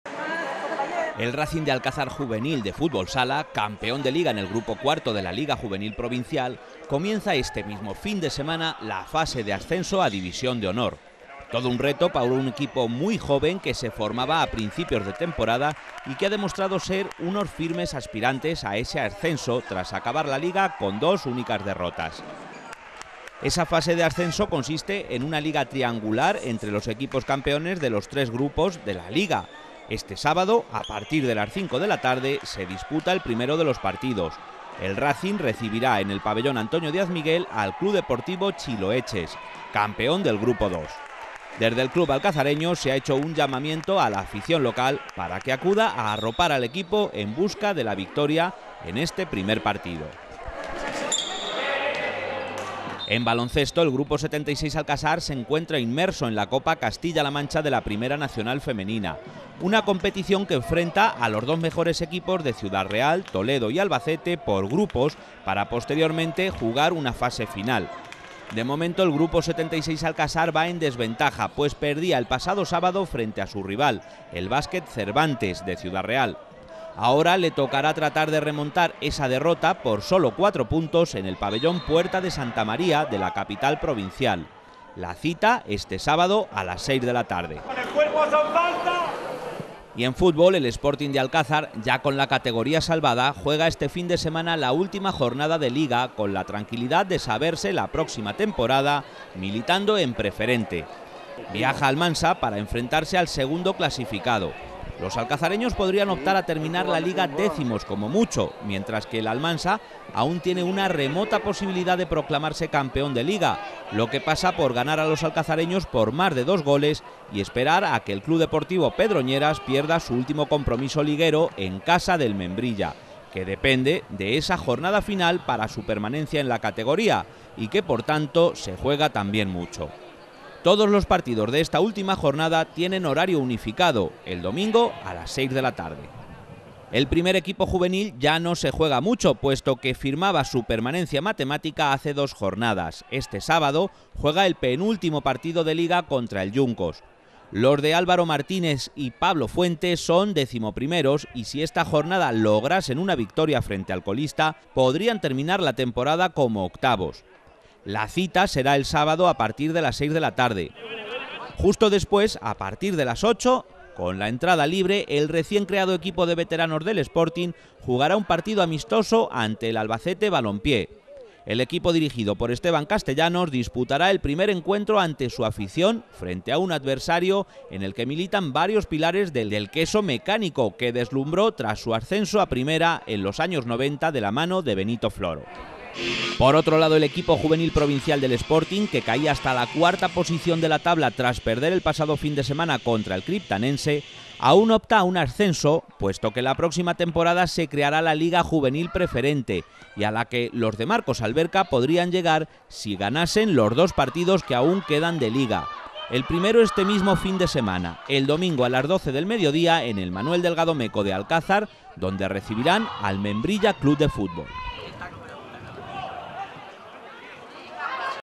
Los juveniles del Racing se juegan el ascenso
Las ligas llegan a su final y los equipos de Alcázar que aún siguen en competición, llegan a este fin de semana con poco en juego ya en la mayoría de los casos. Aunque los juveniles del fútbol sala aún son firmes candidatos al ascenso de categoría.